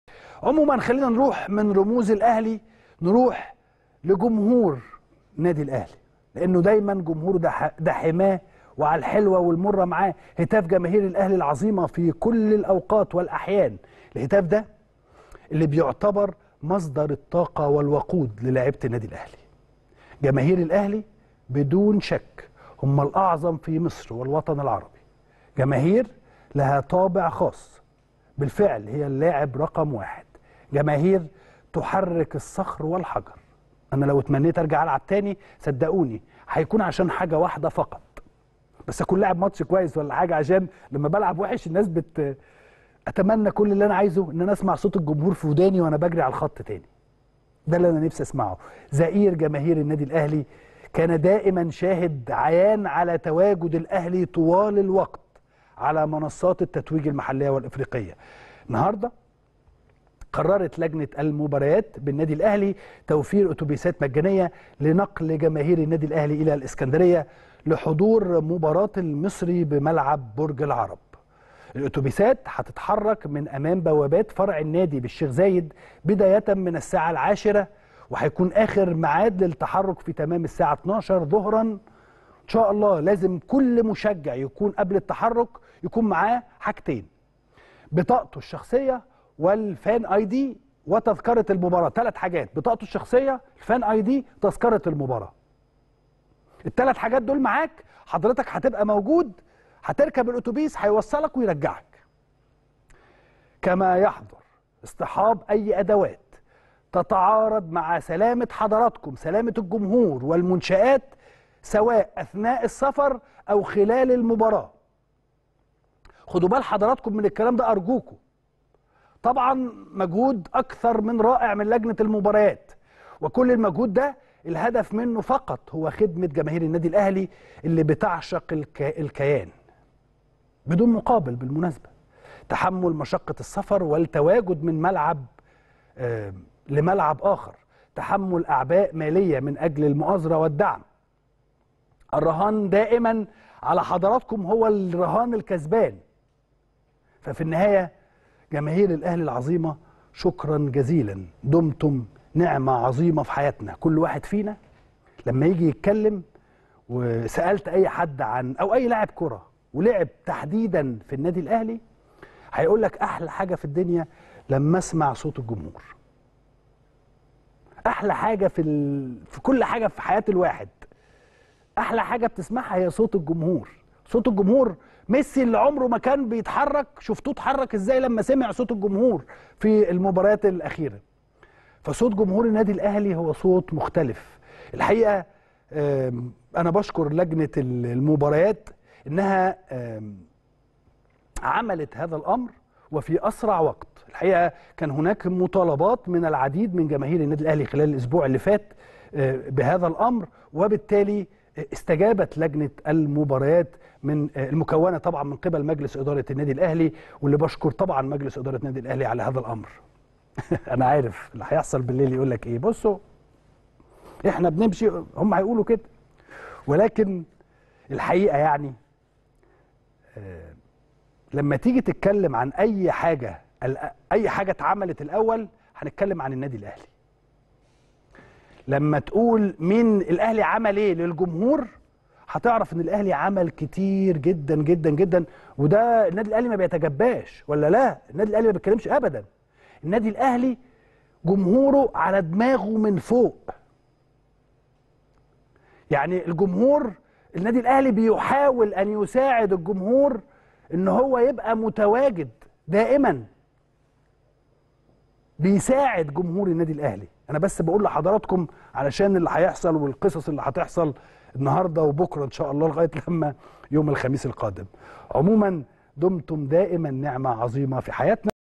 الشاطر: وحشني صوت جمهور الاهلي ونفسي ارجع للملعب لسماعه.. وبشكر لجنة المباريات على مجهودهم الرائع.
إسلام الشاطر: وحشني صوت هتاف جمهور الاهلي ونفسي ارجع للملعب لسماعه.. وبشكر لجنة المباريات على مجهودهم لتسهيل حضور جماهيرنا للمباراة القادمة أمام المصري | البيت الكبير